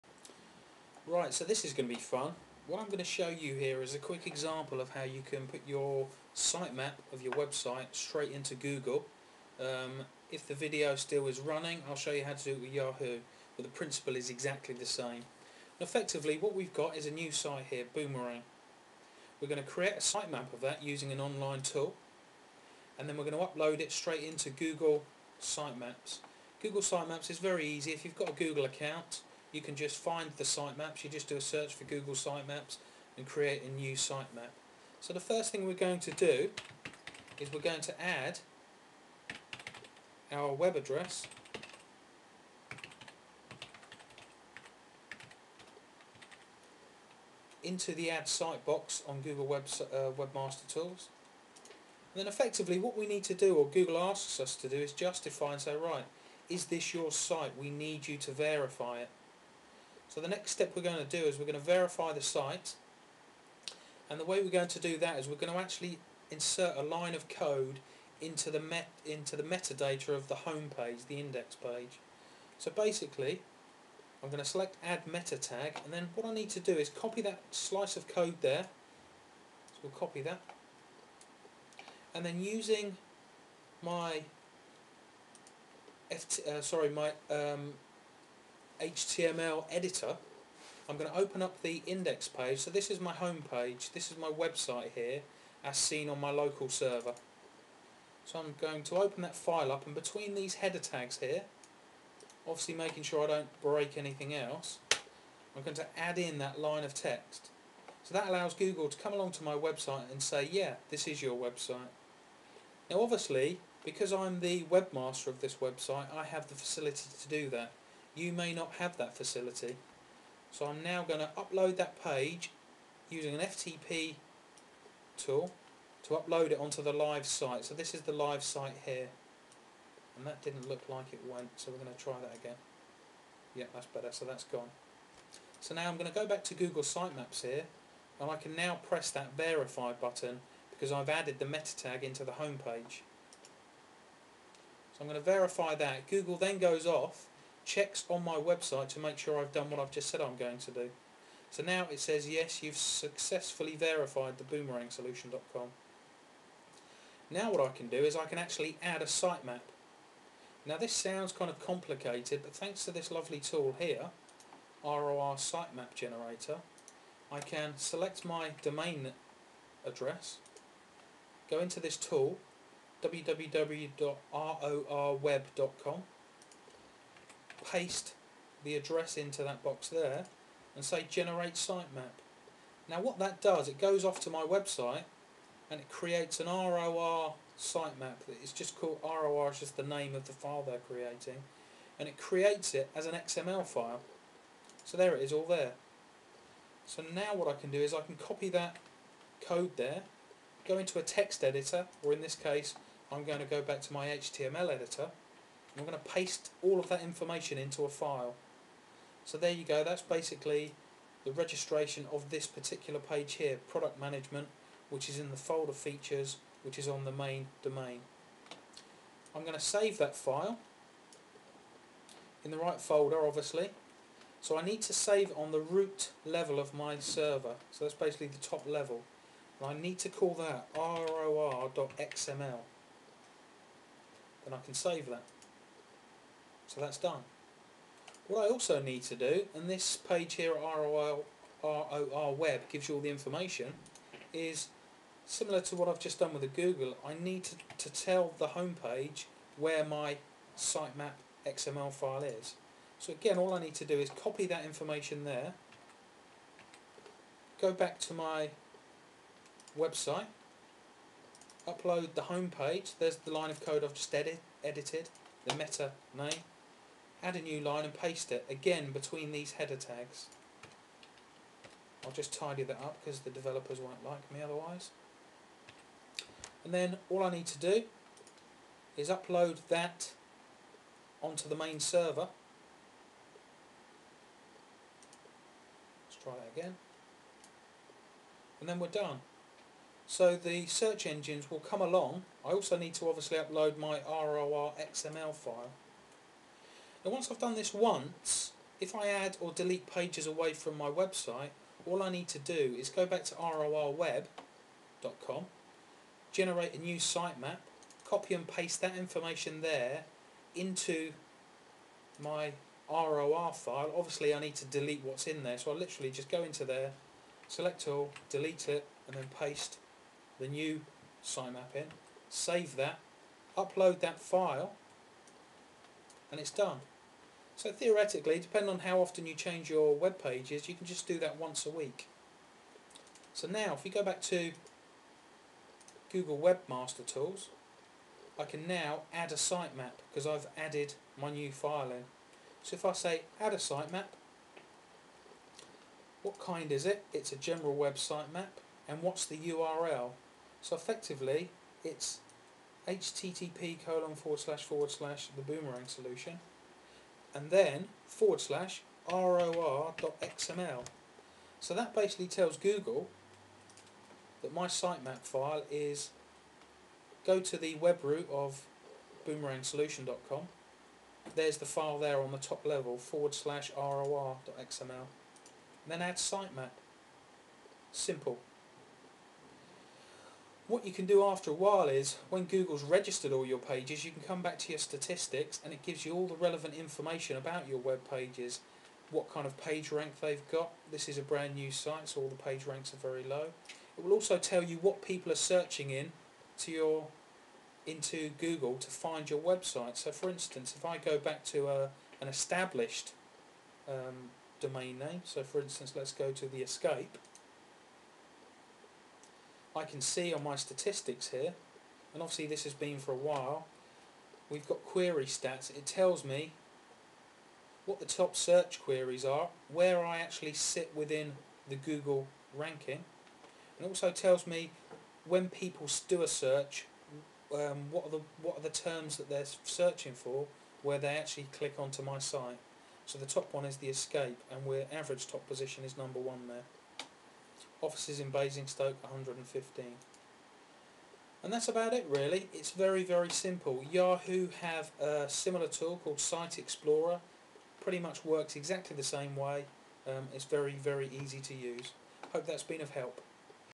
Creating A Google Site Map
Quick basic lesson in how to create a sitemap using online tools, and upload it straight into Google Sitemaps.